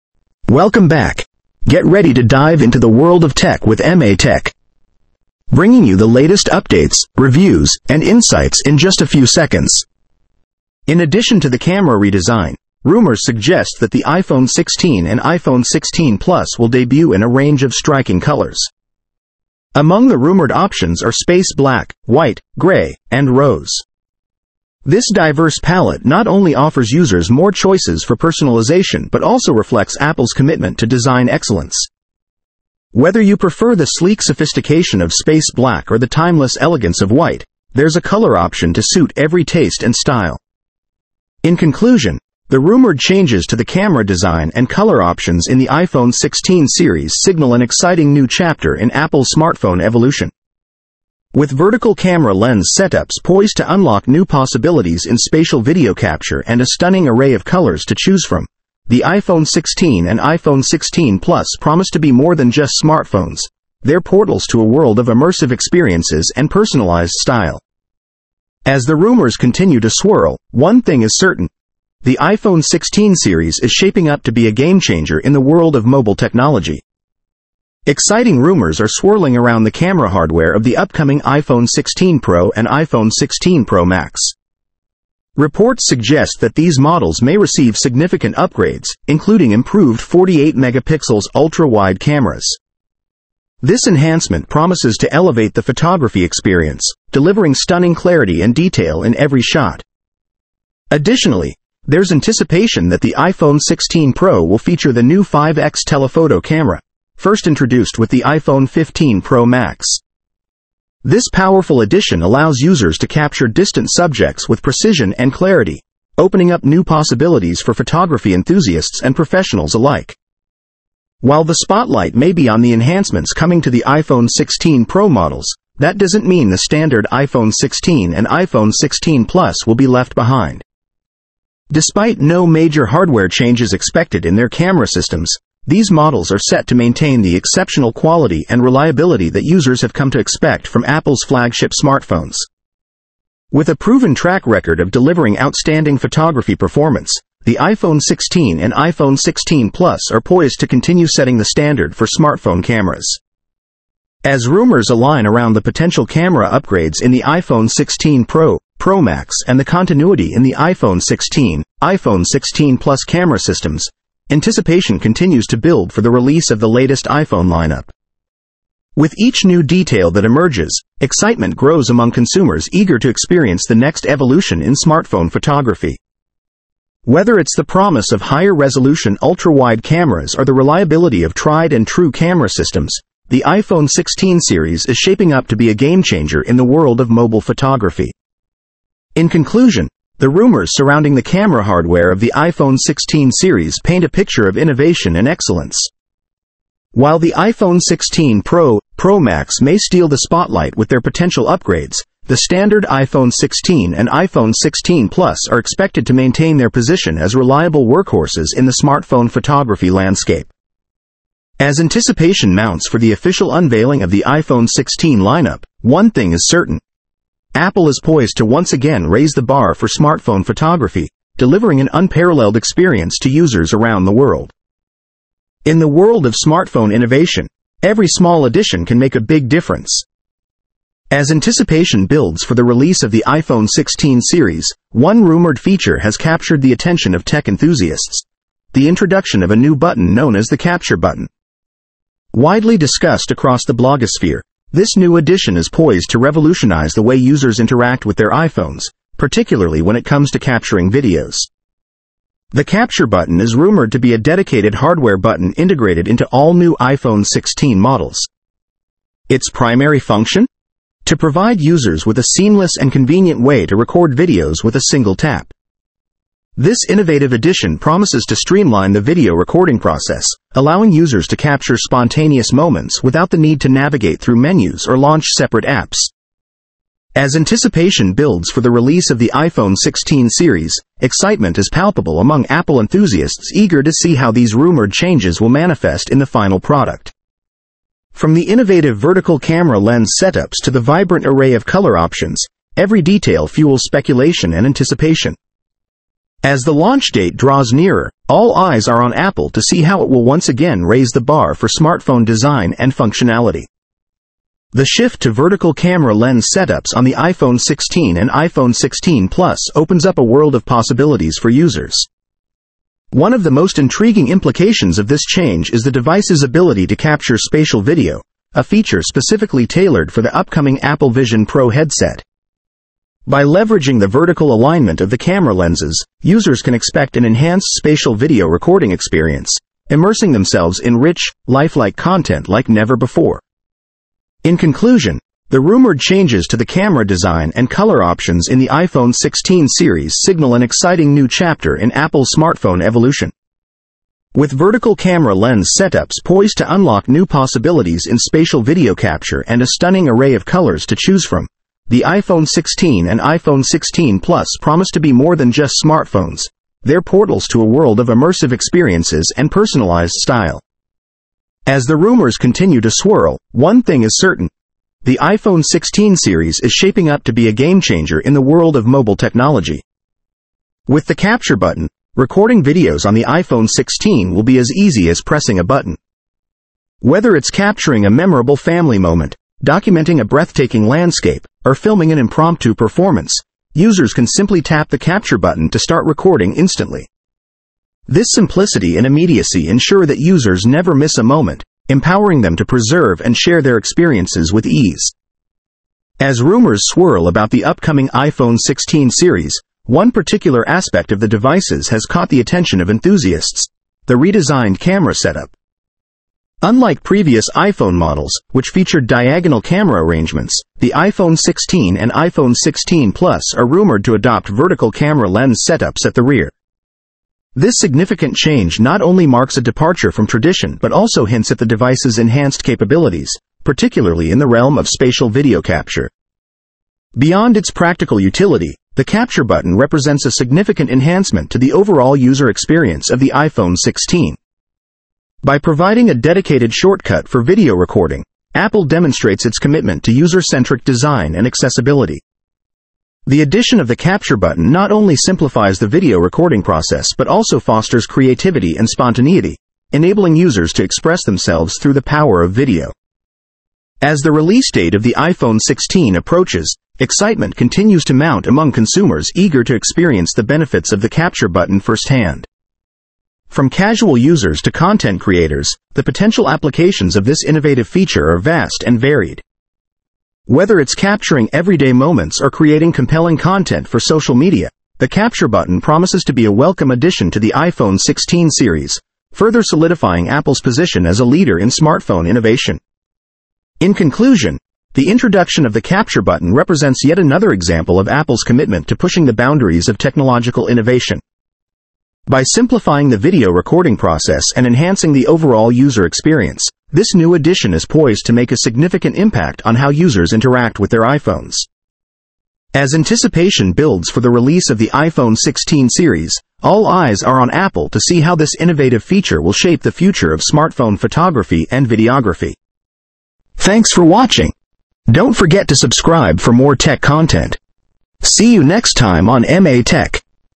iPhone 16 Series Design, Features, and Innovations !!!!!.......
"iPhone 16 Series: Design, Features, and Innovations! 📱✨ Dive into the remarkable design, advanced features, and groundbreaking innovations of the iPhone 16 Series. Explore the cutting-edge specifications and technology that set this series apart. Discover how Apple continues to push the boundaries of smartphone design and innovation, creating the future of mobile technology. Experience the ultimate in design and functionality with the iPhone 16 Series!iPhone16Series,